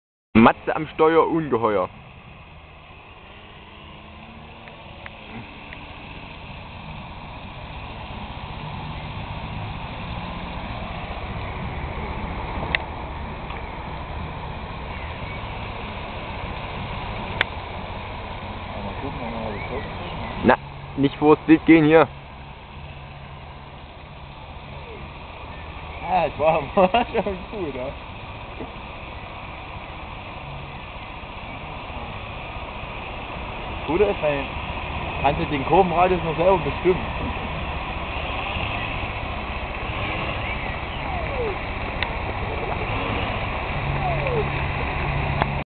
HydroFoam - 1. Run Matze
Our first run on water with the brand new HydroFoam.
Pilot: Matze